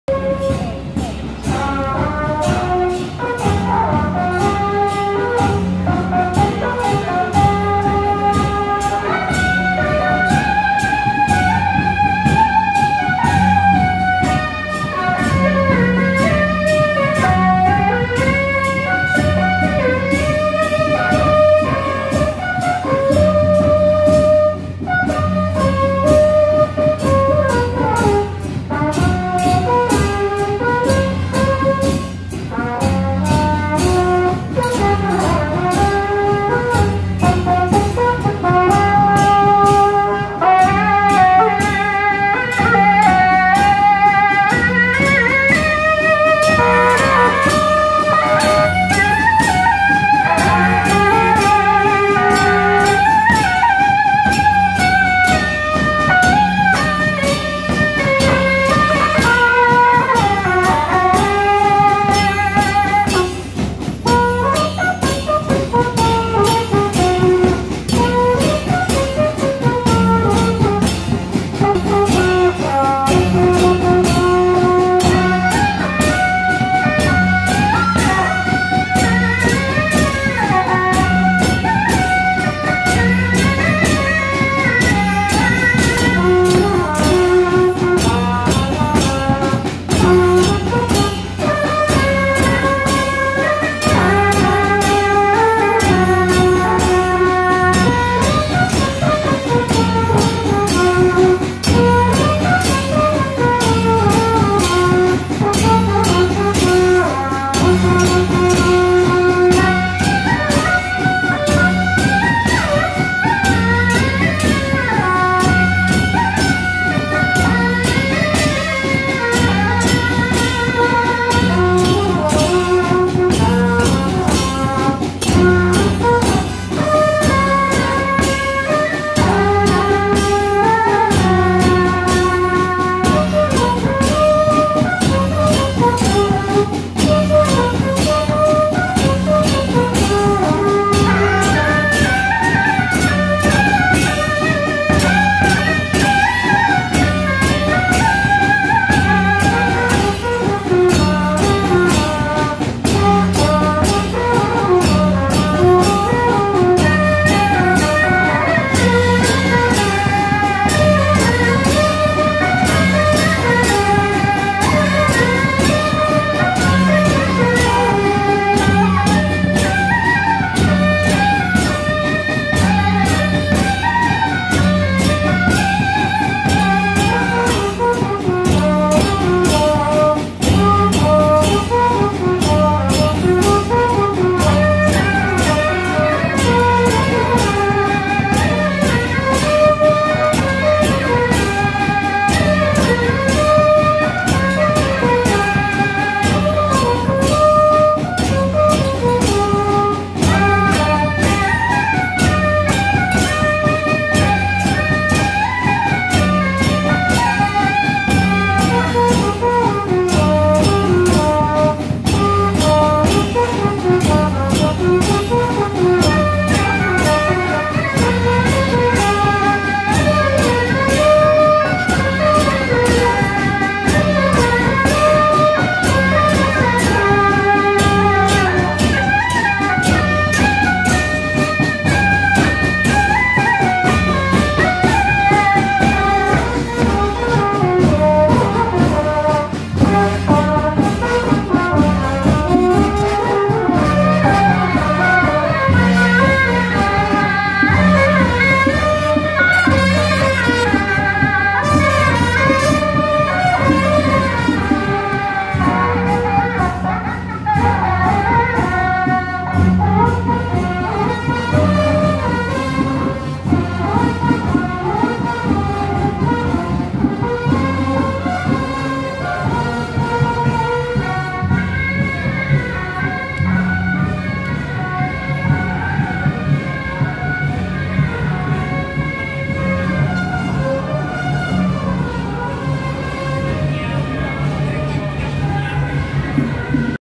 【羅東嘉興團-中西樂】~111板橋後埔福德宮福德正神建宮160週年紀念遶境
羅東嘉興團-中西樂